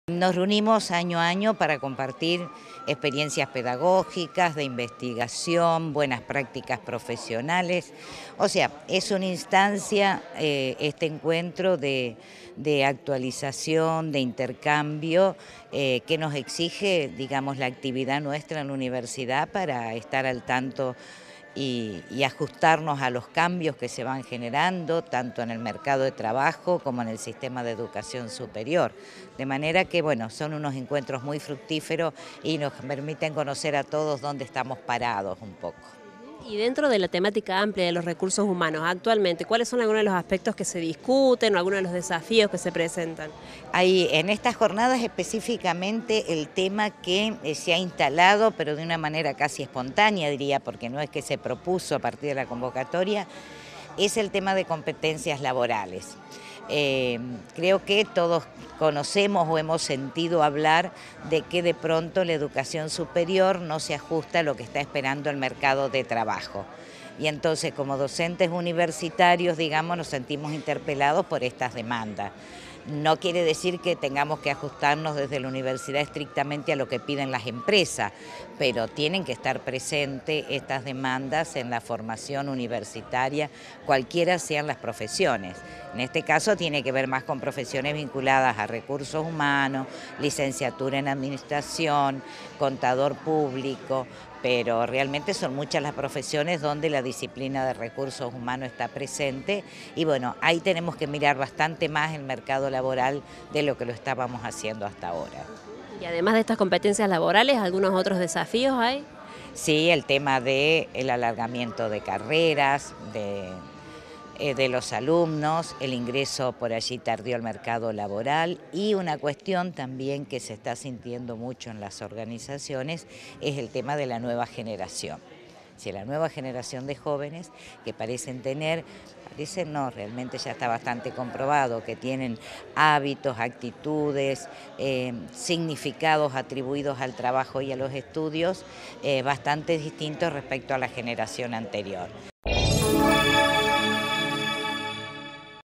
JORNADA SOBRE RECURSOS HUMANOS - NORMA ZANDOMENI – PRESIDENTA APUARH
DOCENTES E INVESTIGADORES DEBATEN SOBRE EXPERIENCIAS Y DESAFÍOS EN EL ÁREA
Facebook.com/uniriotv
Twitter.com/uniriotv